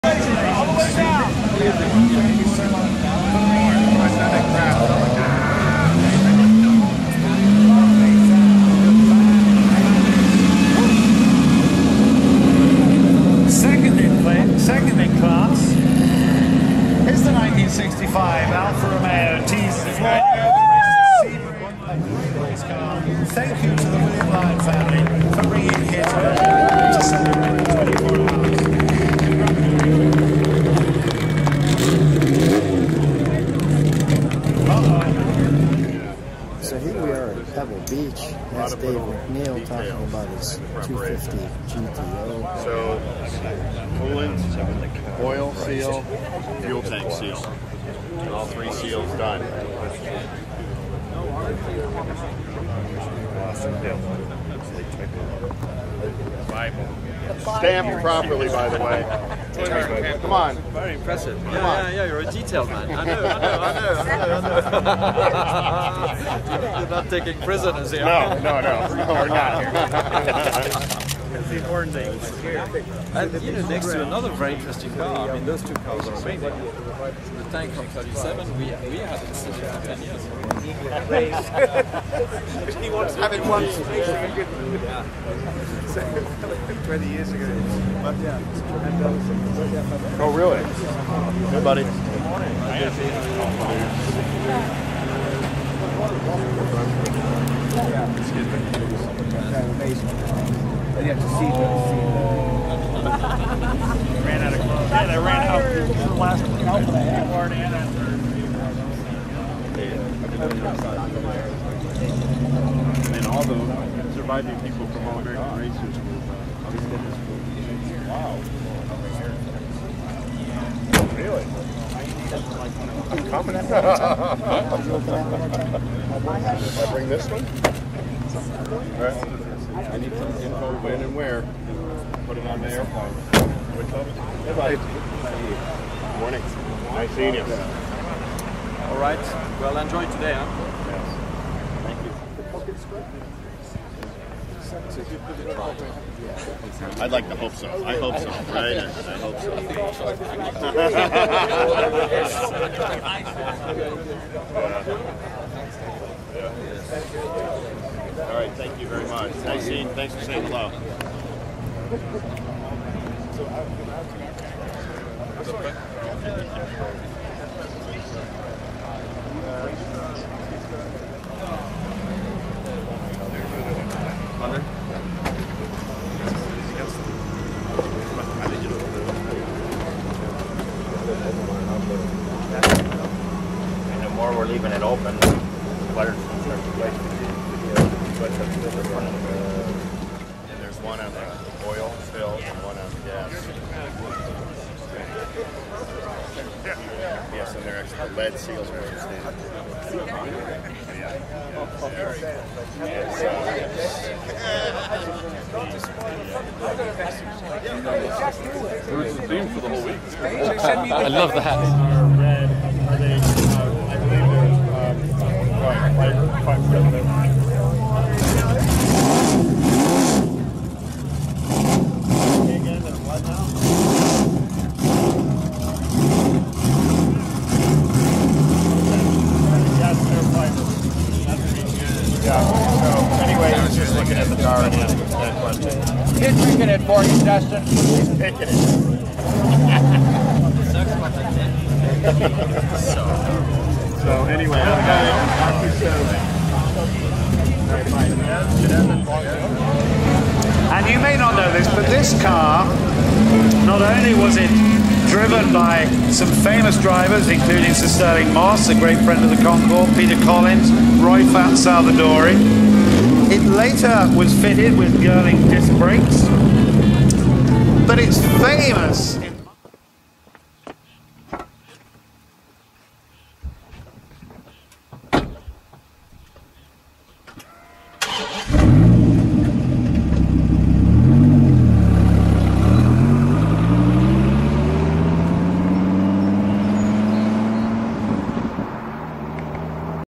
1962 Ferrari 250 GTO # 4153 GT in Silver & V12 Engine Sounds on My Car Story with Lou Costabile
On "My Car Story" we're at the Pebble Beach Concours d’Elegance in Pebble Beach CA on 8-21-22.

We're looking at a 1962 Ferrari 250 GTO Scaglietti Berlinetta # 4153 GT in Silver Metallic Paint and a French Tricolor stripe of Blue, White and Red. The car comes with the factory Tipo 168/62 Comp V-12 engine, and a manual transmission.

The car's Owner is David MacNeil. This car in 1964 was the overall victory Winner at the Tour de France driven by Lucien Bianchi & Georges Berger. This car is the 24th of 36 GTO’s produced. I do not have hard evidence, however when researching on the internet from “DuPont Registry” & “The Drive” & “CNN Style” this car was sold and purchased in 2018 in a private sale for $70-80 million dollars.